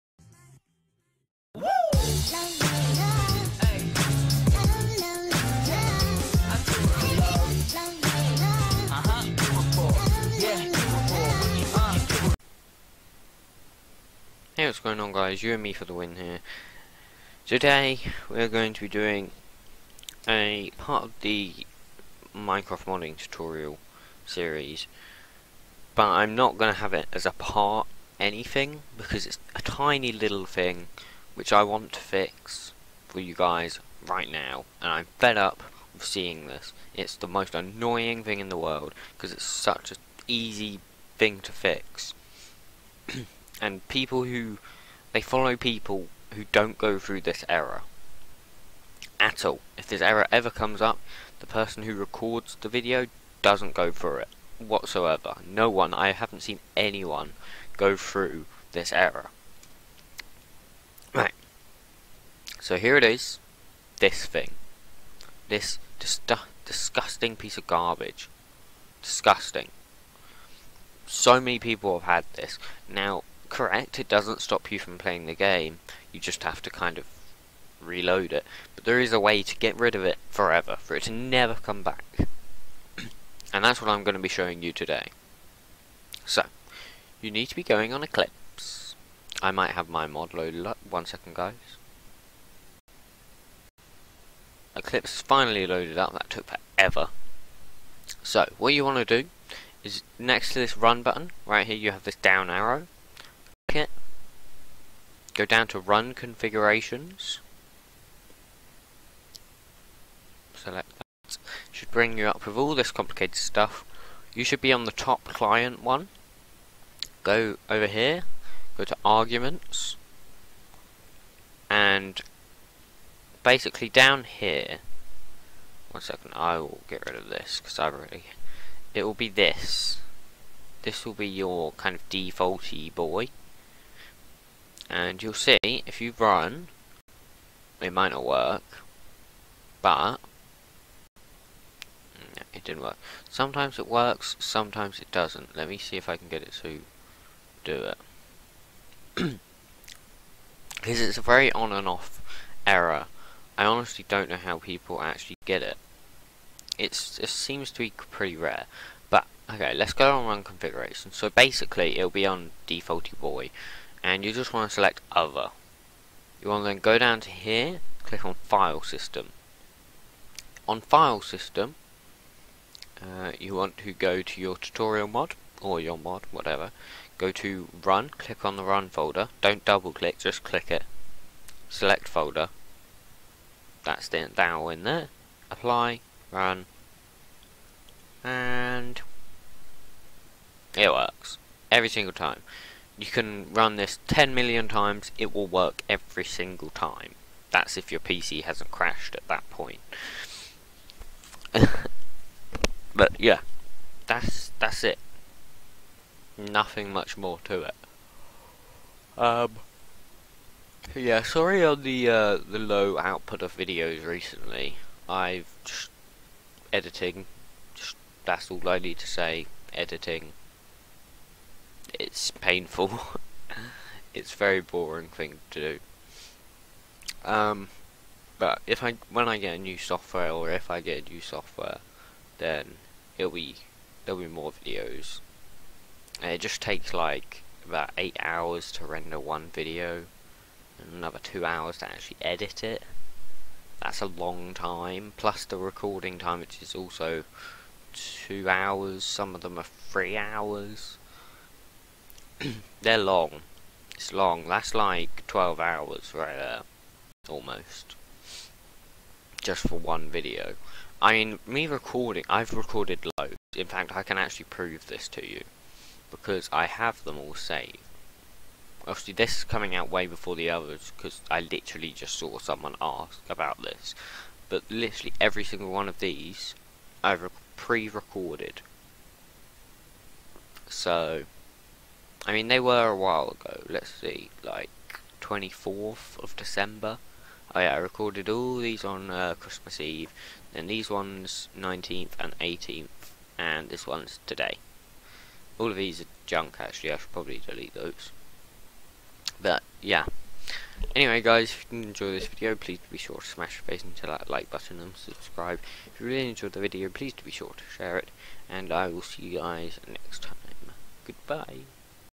Minecraft Modding 1.12.2 Tutorial - Fixing the Launch Client Error
Hey guys, sorry that I haven't been posting in a while, I haven't had the push to want to edit and upload. Also soon I am going to be getting a new microphone soon which hopefully won't pick up the background sound as that is rather overpowering the video. Hopefully I will be able to get some more videos out now such as Episode 8.